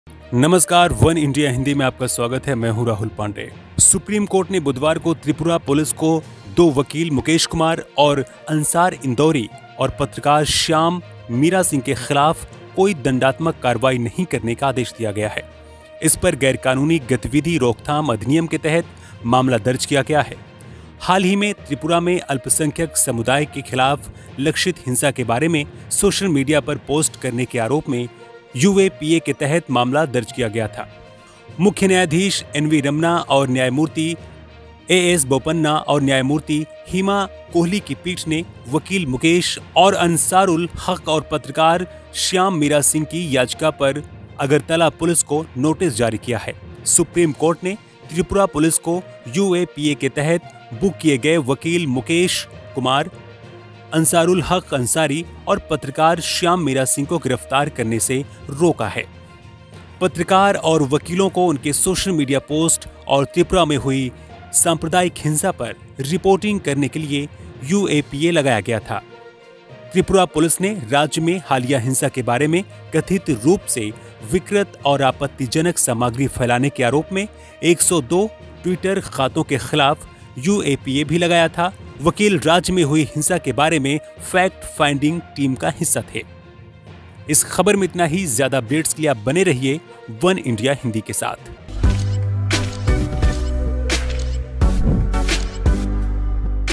Tripura Violence: वकील और पत्रकारों बड़ी राहत, SC से गिरफ्तारी पर रोक | वनइंडिया हिंदी
The Supreme Court on Wednesday ordered Tripura Police not to take any coercive action against two lawyers Mukesh Kumar and Ansar Indori and journalist Shyam Meera Singh. What is the whole matter this report

Supreme Court ने Wednesday को Tripura Police को दो वकील Mukesh Kumar और Ansar Indori और पत्रकार Shyam Meera Singh के खिलाफ कोई दंडात्मक कार्रवाई नहीं करने का आदेश दिया है. क्या है पूरा मामला ये रिपोर्ट देखिए.